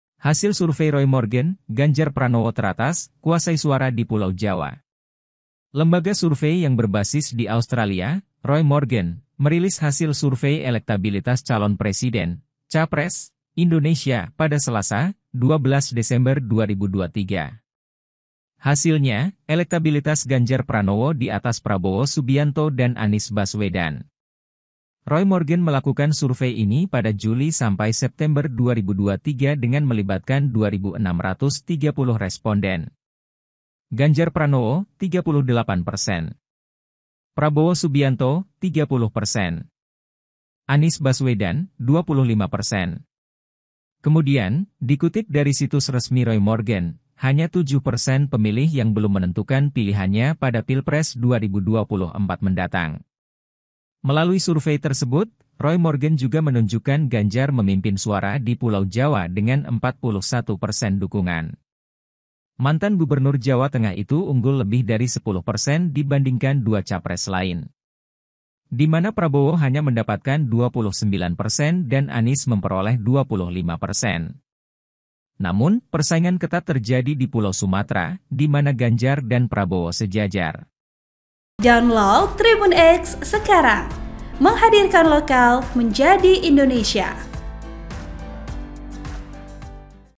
Hasil Survei Roy Morgan: Ganjar Pranowo Teratas, Kuasai Suara di Pulau Jawa
TRIBUNNEWS.COM - Lembaga survei yang berbasis di Australia, Roy Morgan, merilis hasil survei elektabilitas calon presiden (capres) Indonesia pada Selasa (12/12/2023).

Hasilnya, elektabilitas Ganjar Pranowo di atas Prabowo Subianto dan Anies Baswedan.

Roy Morgan melakukan survei ini pada Juli sampai September 2023 dengan melibatkan 2.630 responden.

- Ganjar Pranowo: 38 persen

- Prabowo Subianto: 30 persen

- Anies Baswedan: 25 persen

Kemudian, dikutip dari situs resmi Roy Morgan, hanya tujuh persen pemilih yang belum menentukan pilihannya pada Pilpres 2024 mendatang.

Melalui survei tersebut, Roy Morgan juga menunjukkan Ganjar memimpin suara di Pulau Jawa dengan 41 persen dukungan.

Mantan Gubernur Jawa Tengah itu unggul lebih dari 10 persen dibandingkan dua capres lain.

Di mana Prabowo hanya mendapatkan 29 persen dan Anies memperoleh 25 persen.

Namun, persaingan ketat terjadi di Pulau Sumatra, di mana Ganjar dan Prabowo sejajar.

VP: TEGAR